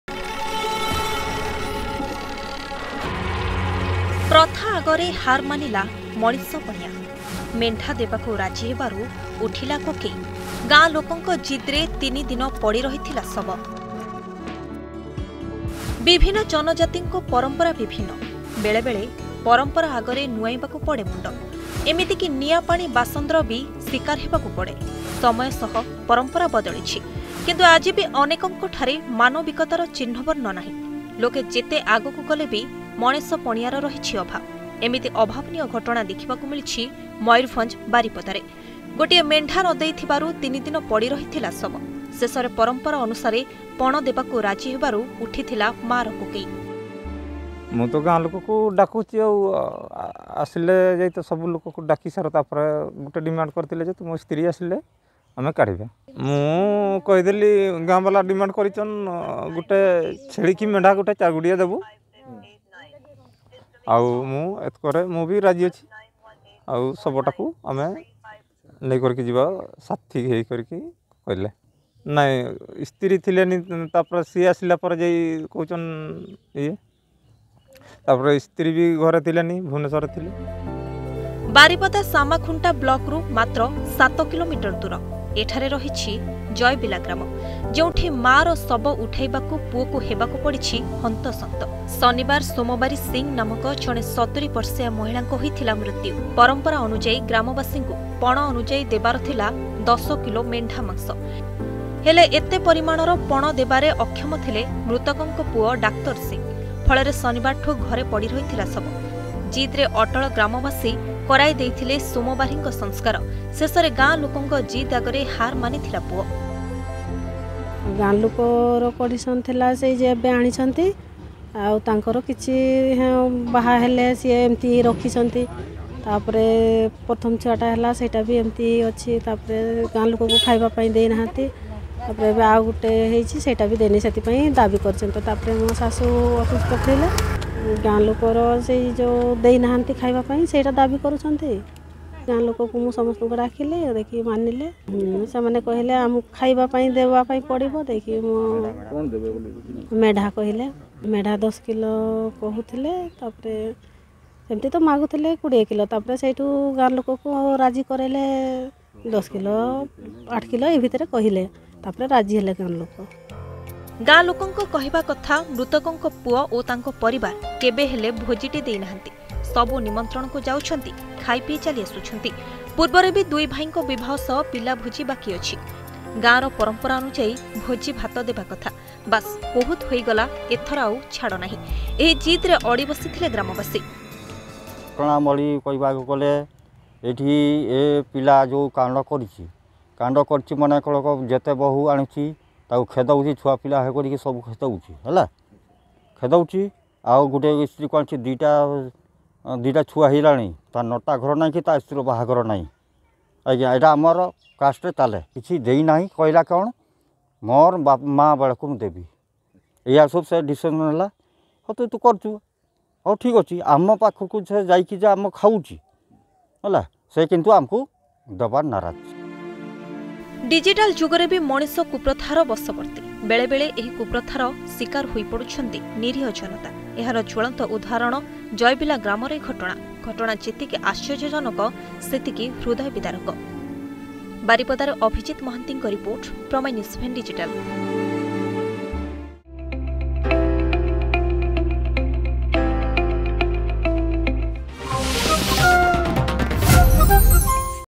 ପ୍ରଥା ନା ଅନ୍ଧବିଶ୍ୱାସ ! ପରମ୍ପରା ଆଗରେ ନଇଁଲା ମଥା, ମେଣ୍ଢା ଦେବାରୁ ଉଠିଲା କୋକେଇ ..
PrameyaNews7, is a leading Odia News Channel of Odisha, India. Prameya News7 provides Fair, Unbiased news from Odisha and beyond.